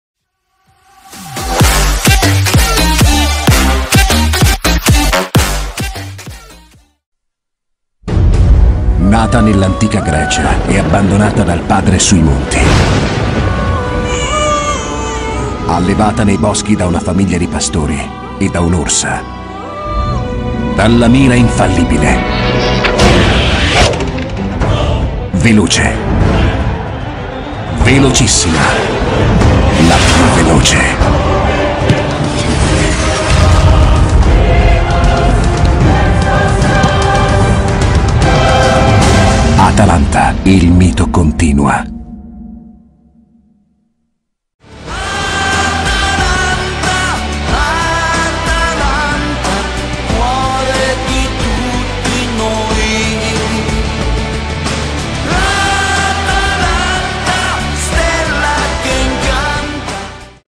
La Dea Atalanta - Il Mito
La storia dietro alla nostra fantastica Dea in un video!